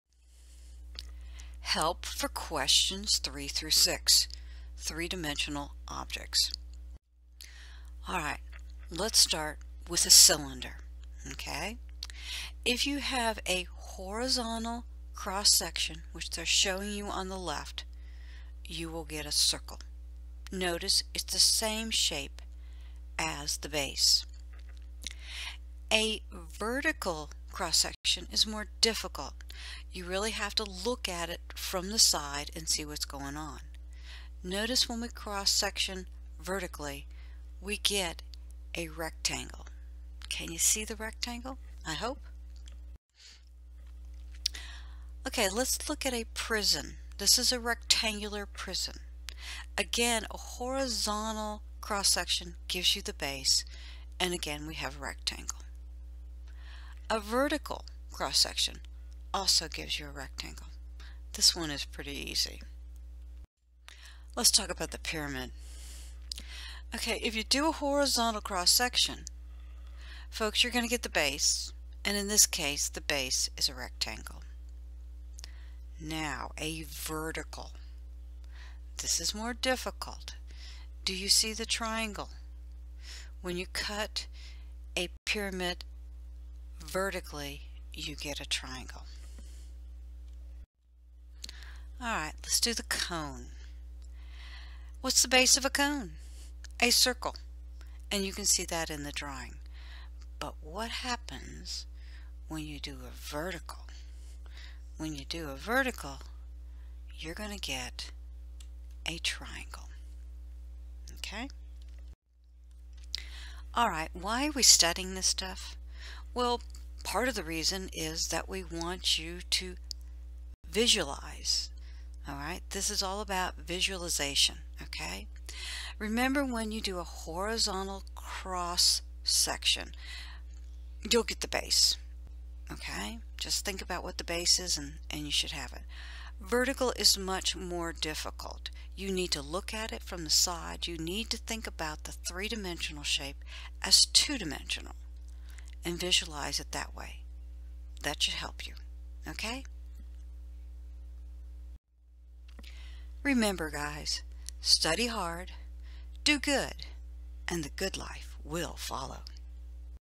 Cross Sections of 3D Objects
What are the Cross Sections of 3D objects. Review for Georgia Analytic Geometry, Unit 3 Test.